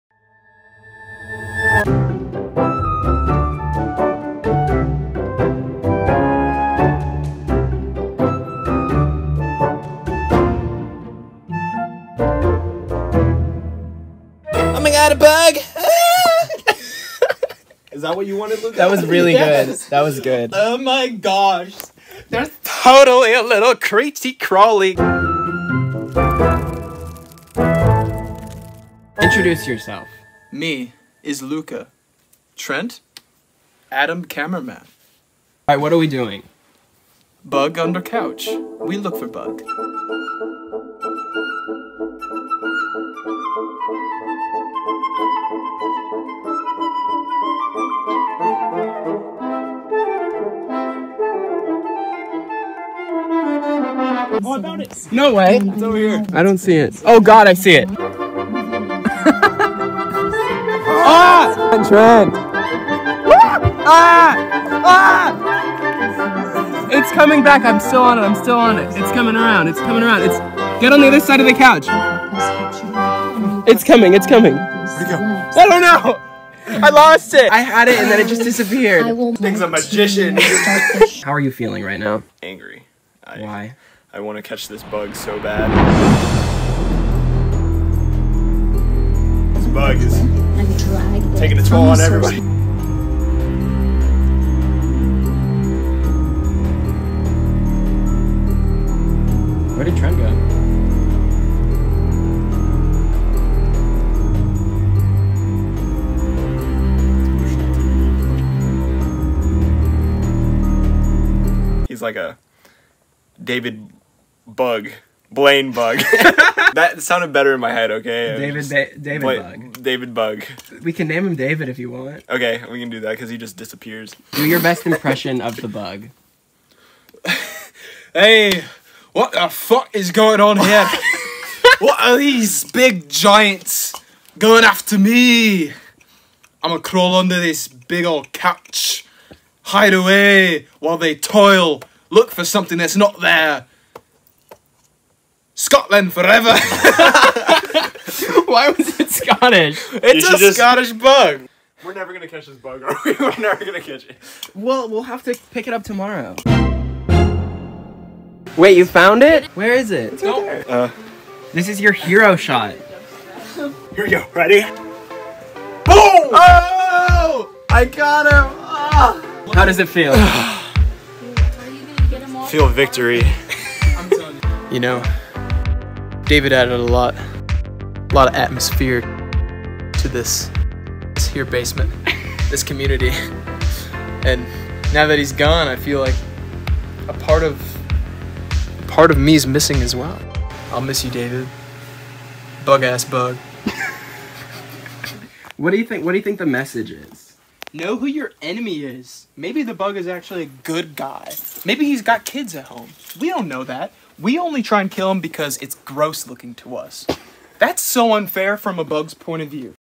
Bug Ass Bug a bugumentary
I hope you enjoy this bugumentary where me and the boys track down David Bug, a centipede that was hiding under my couch.

Bugscribe.